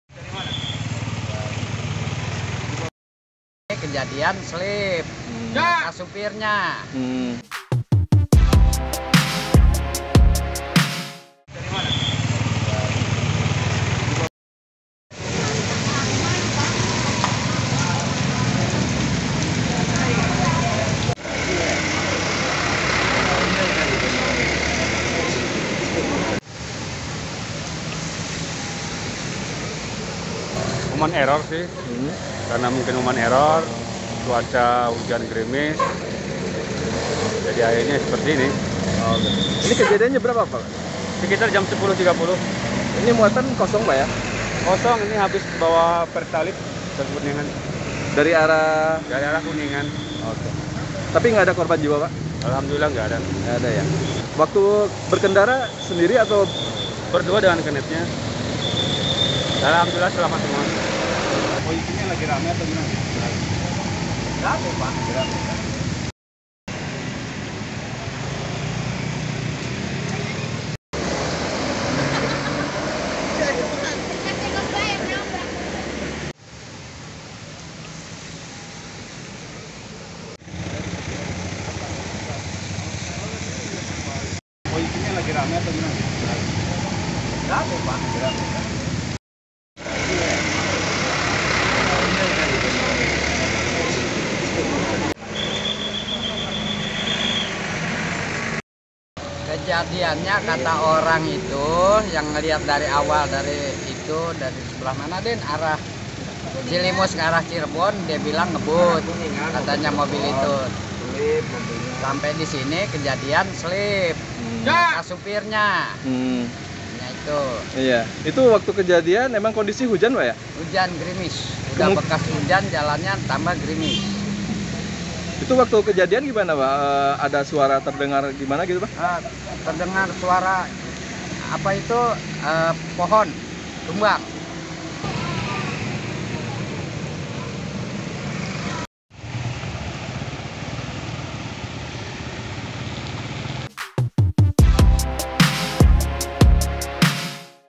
Ban Slip, Jalanan Licin, Tangki Hilang Kendali di Beber
Kecelakaan tunggal terjadi di jalur Cirebon-Kuningan. Tepatnya di Desa Halimpu, Kecamatan Beber, Kabupaten Cirebon. Kejadian Selasa (8/2/2022) sekitar pukul 10.00 WIB. Dugaan sementara, kendaraan alami slip ban di jalanan menurun. Ditambah kondisi hujan, jalanan jadi semakin licin. Sehingga truk tidak terkendali dan menabrak pohon. Pohon yang tumbang ke badan jalan sempat membuat jalur Cirebon-Kuningan tersendat. Tidak ada korban jiwa dalam kejadian tersebut.